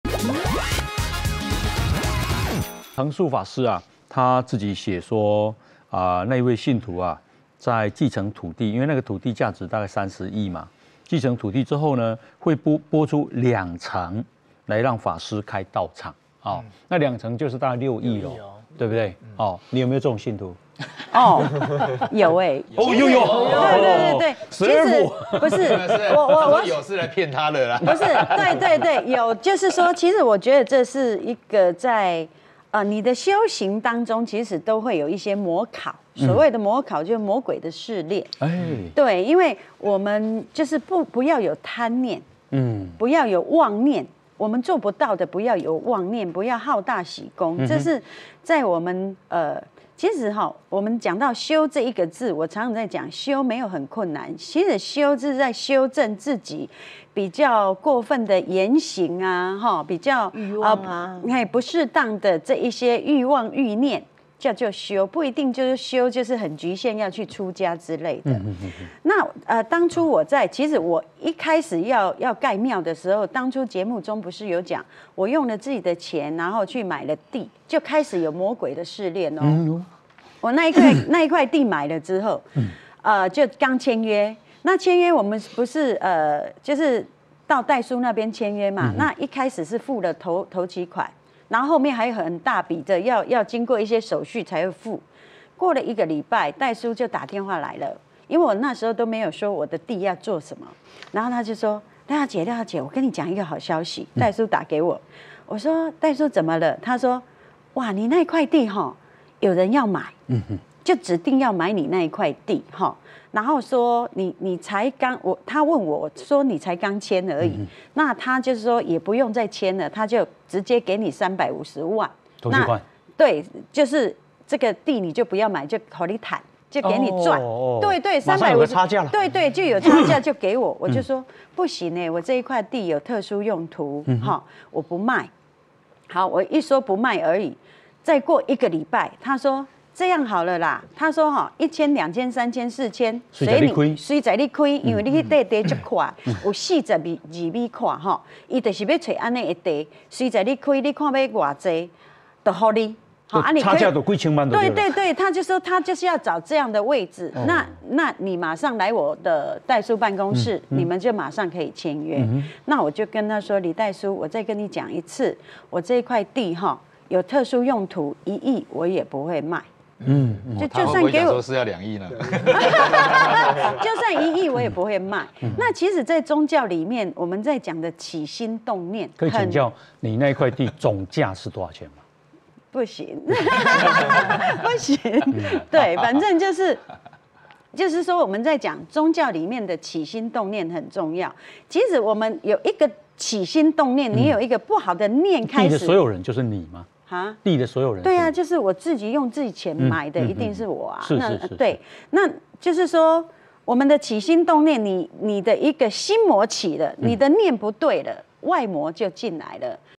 【精華版】出家人心魔誘惑？廖美然30億元魔鬼試煉!
修道之人廖美然說出魔鬼試煉真相！恆述法師被30億心魔誘惑？
▶66歲陳時中爆肝不成人形！
▶許常德痛罵陳超明廢物！